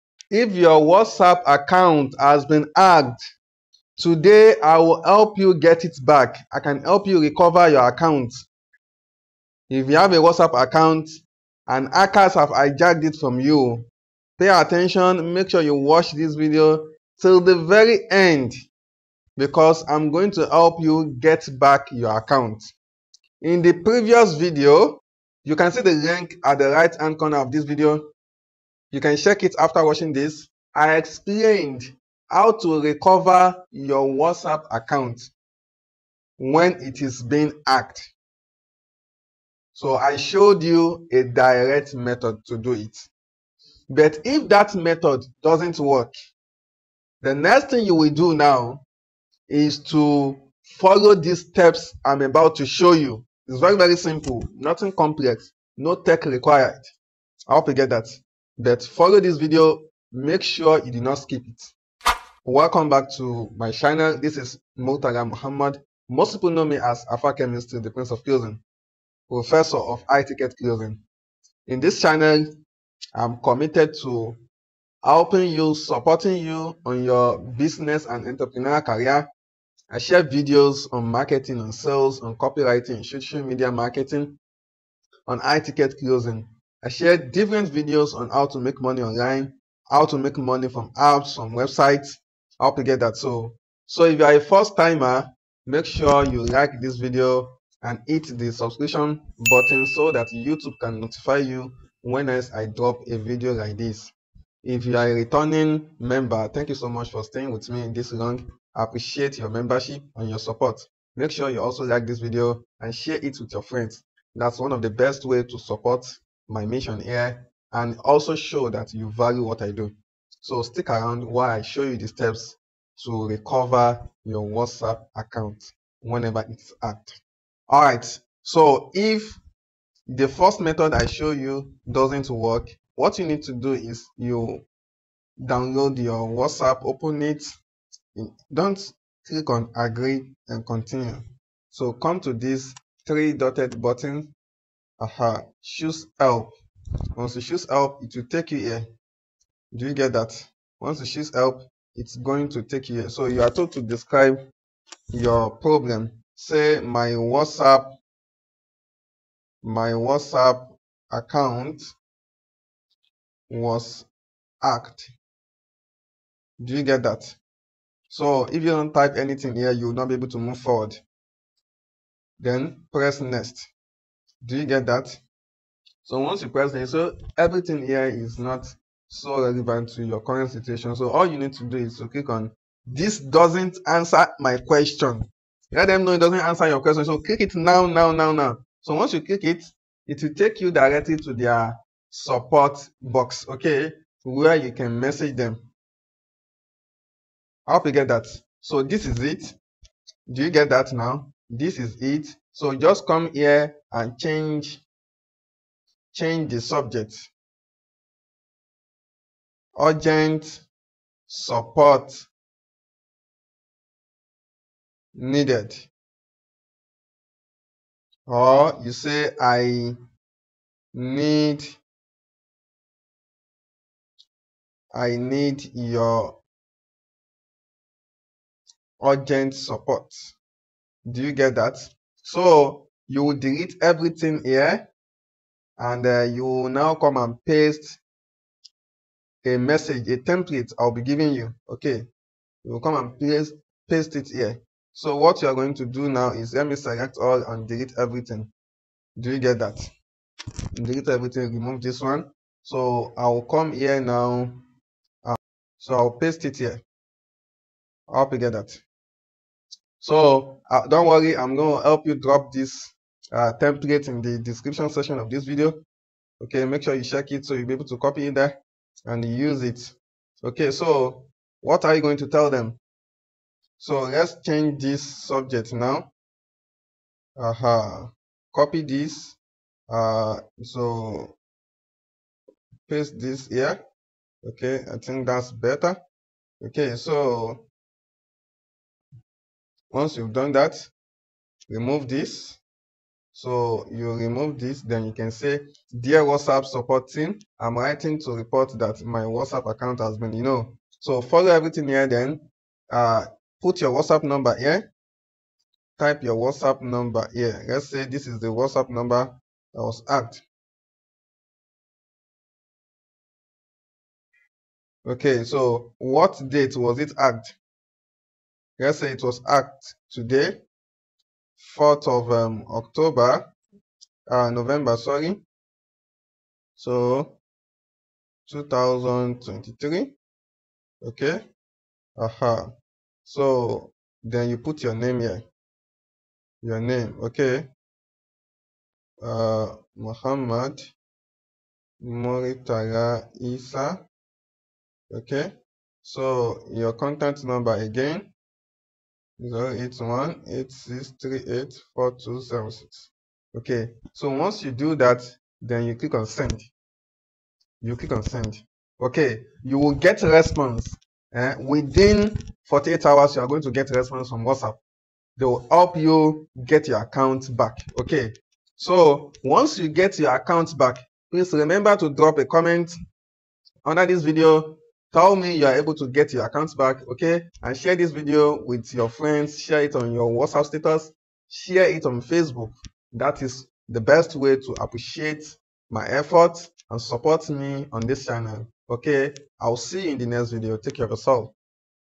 WhatsApp Account Hacked? Recover It in 5 Minutes (Step-by-Step)
This video will help you on how to recover hacked WhatsApp account by yourself. The steps to follow are very easy and it doesn't take time at all.

Subject: Report of Hacked WhatsApp Account - Request for Account Recovery

Dear WhatsApp Support Team,

I am writing to report that my WhatsApp account has been compromised and hacked by unauthorized individuals. I am extremely concerned about the breach of my personal data and communications.

My registered phone number: [Your phone number]
Date of the incident: [Date when you noticed the hack]

I kindly request your assistance in recovering and securing my WhatsApp account. Any guidance and support you can provide to restore access and safeguard my account's integrity would be greatly appreciated.

Please let me know the necessary steps or procedures to regain control of my account as soon as possible.

Thank you for your prompt attention to this matter. I am looking forward to your guidance and support in resolving this issue.

Best regards,